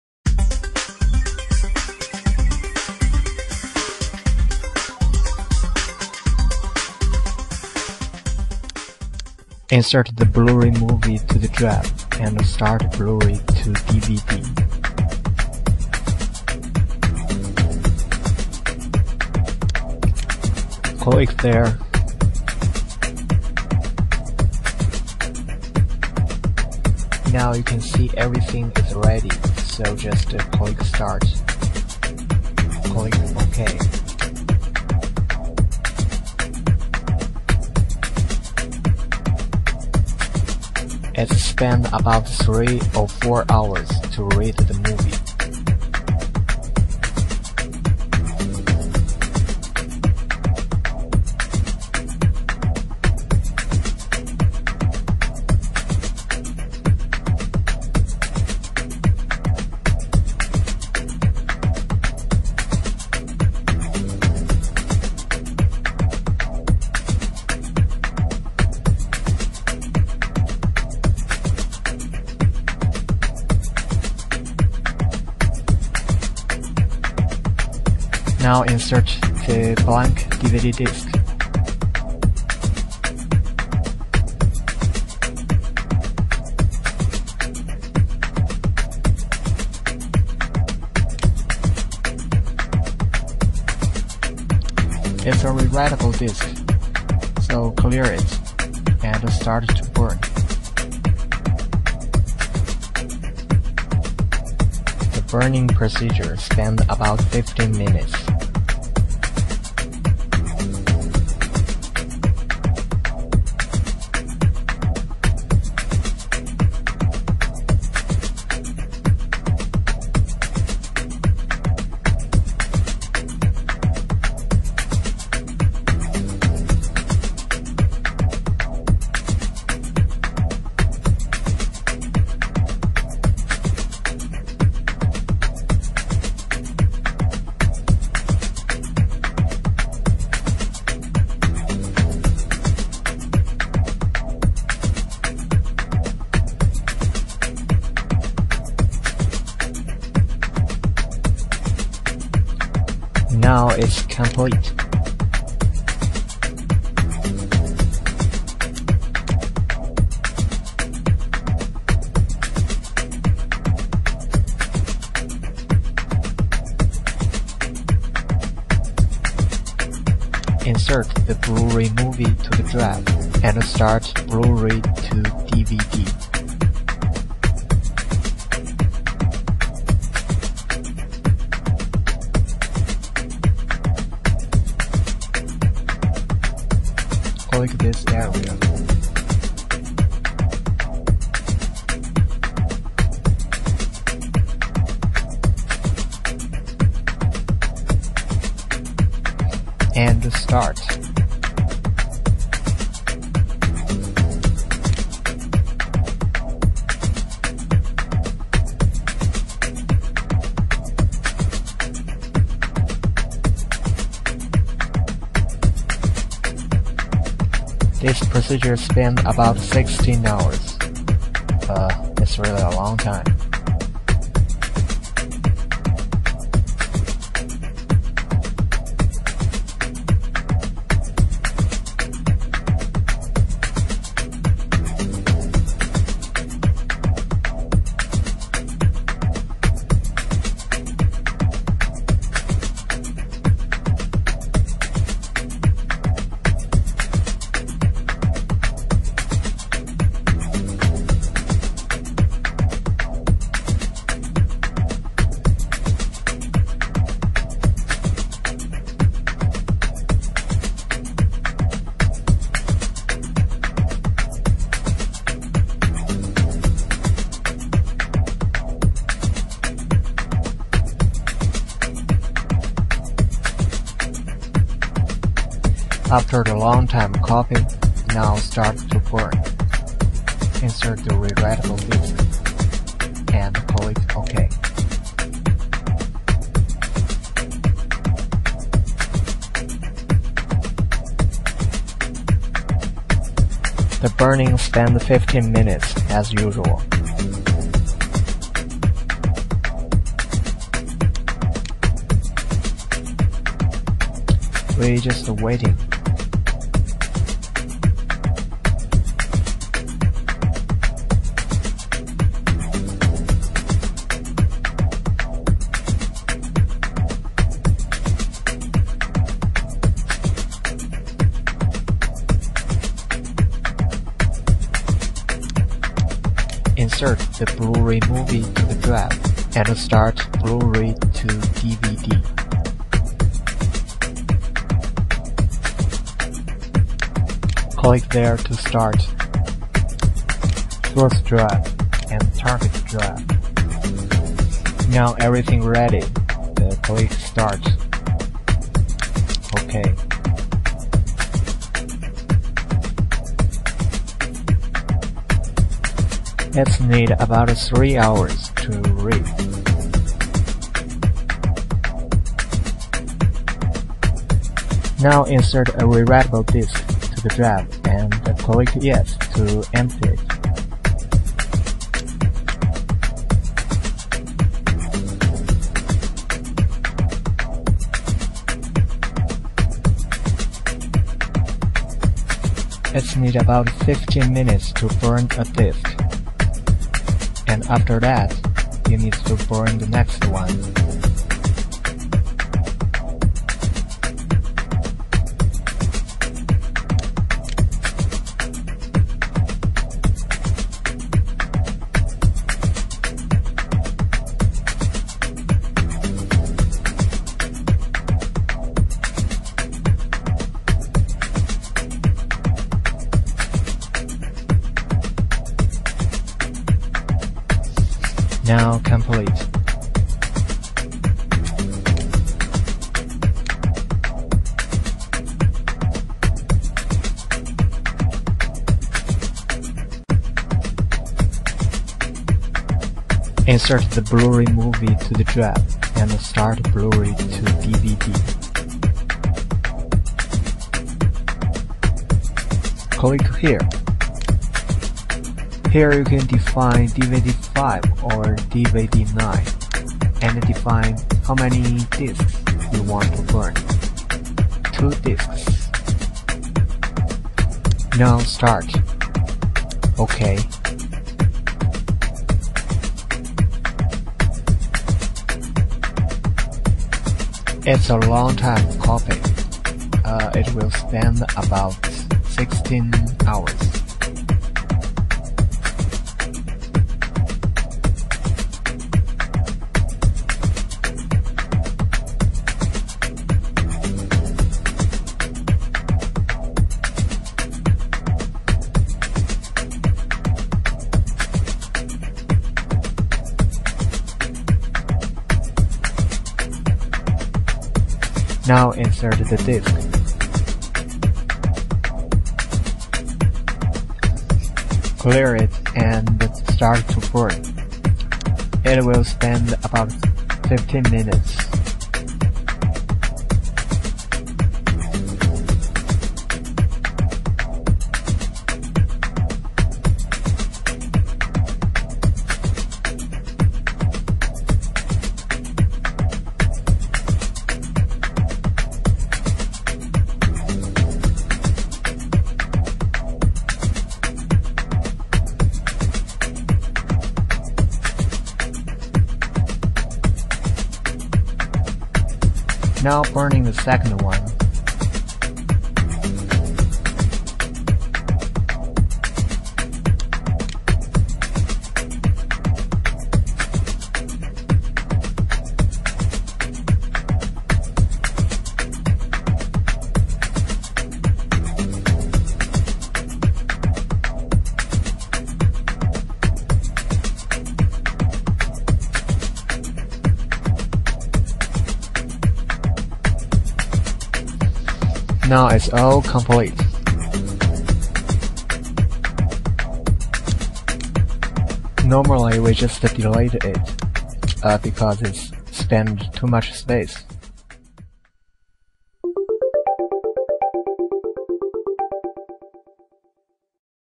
How to Use Blu ray to DVD II Pro
Video demonstrating the blu ray to dvd software.